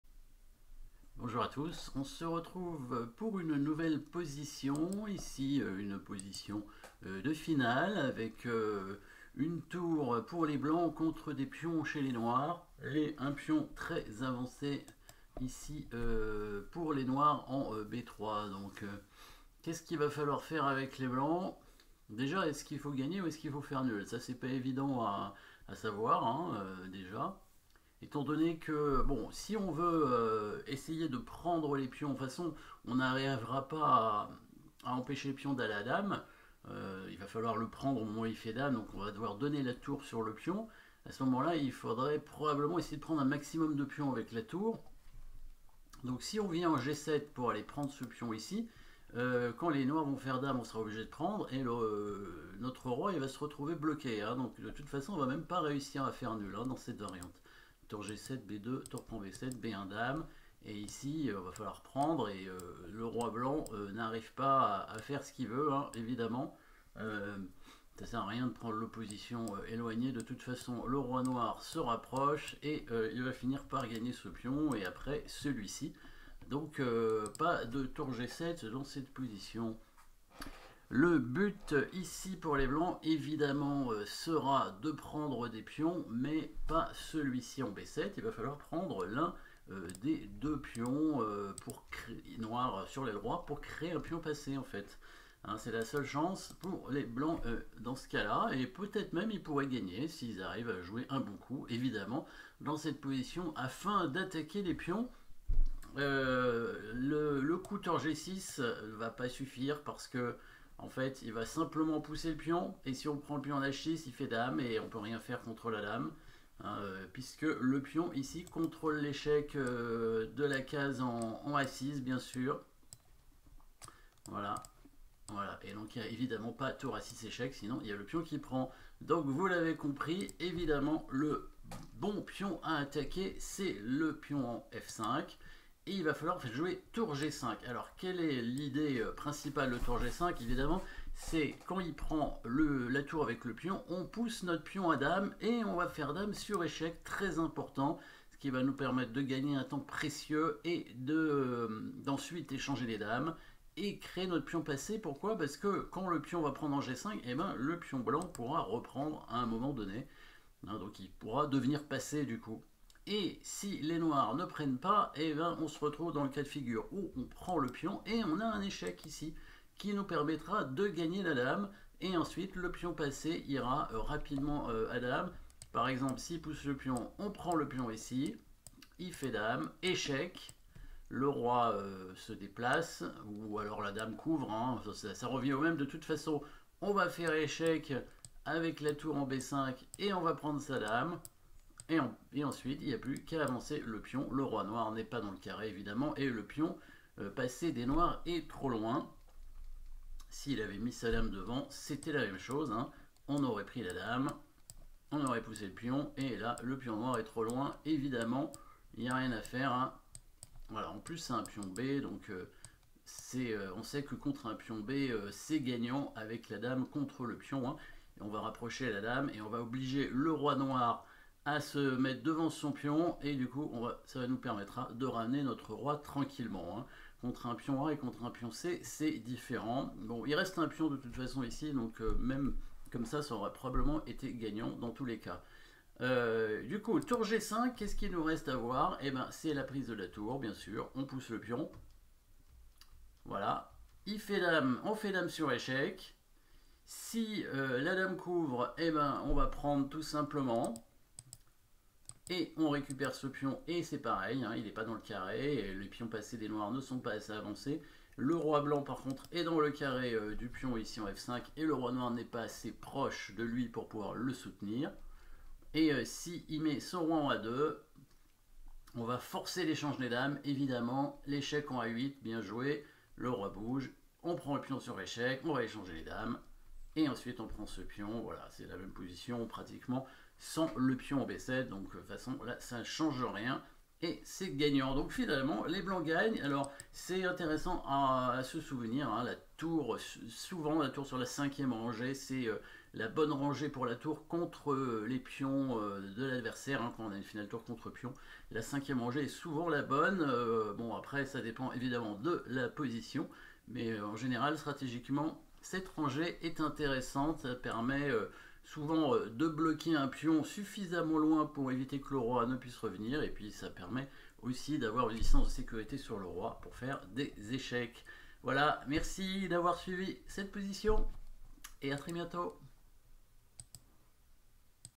Une case cachée
Le pion passé des noirs devrait inévitablement aller à promotion. La tour blanche ira donc se sacrifier dessus à moins que les blancs arrivent à créer de leur côté un pion passé ?
Etude de Kissling 1915